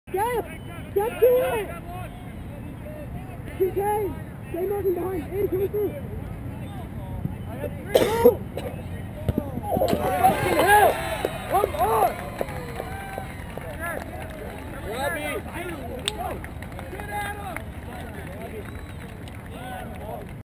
Adam Michaels, Vianney Griffins, scores against Rockhurst in Gateway Soccer Classic
A nice ball in from the right side, about 30 yards out, by Dan Reilly that Michaels runs onto and directs across the face of the goal. Early 2nd half, Rockhurst ended up winning 2-1.